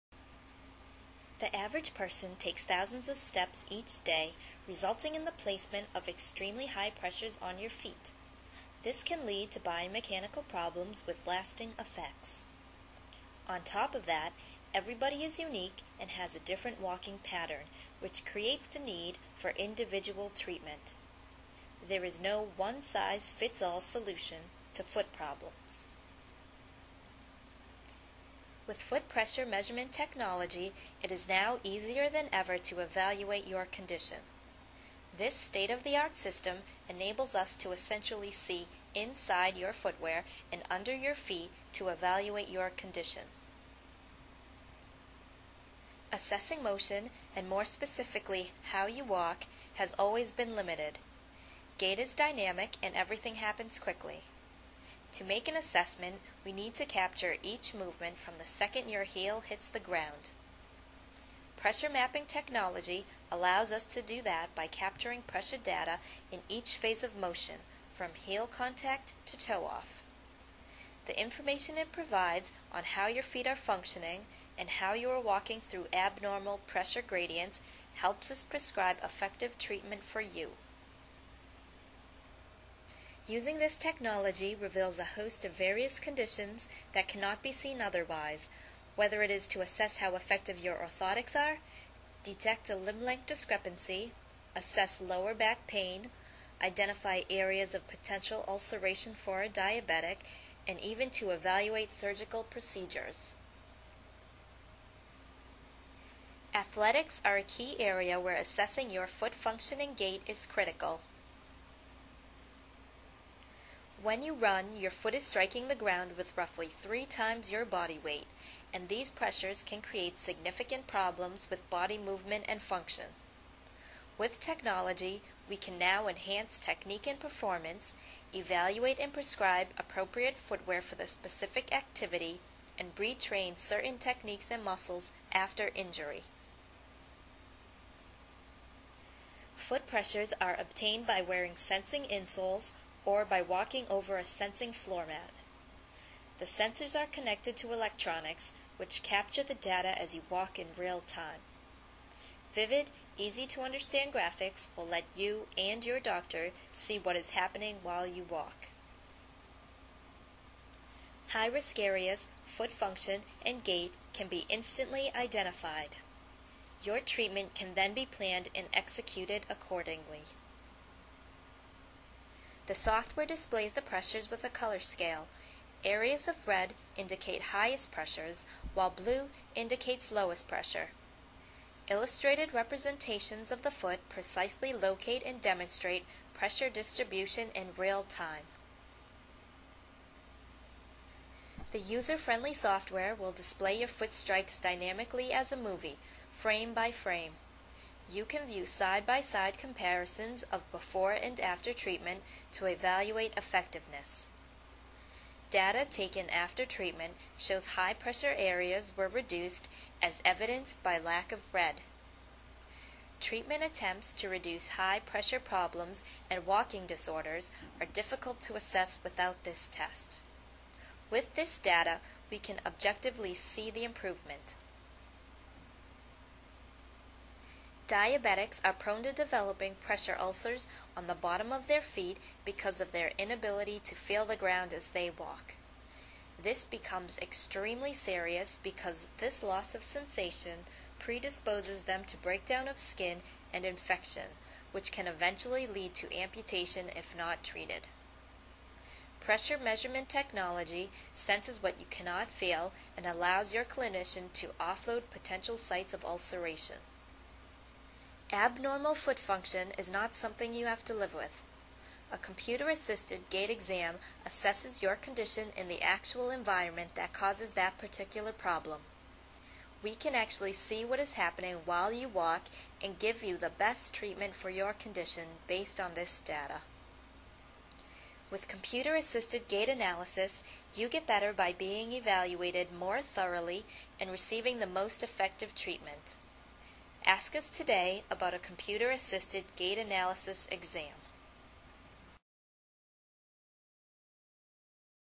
Foot Pressure Mapping Technology for Gait Analysis
View a dynamic presentation on how pressure mapping technology is used for gait analysis and why it is an important part of a patient's treatment. This presentation can also be used in the waiting room to educate your patients about the procedure they are about to have.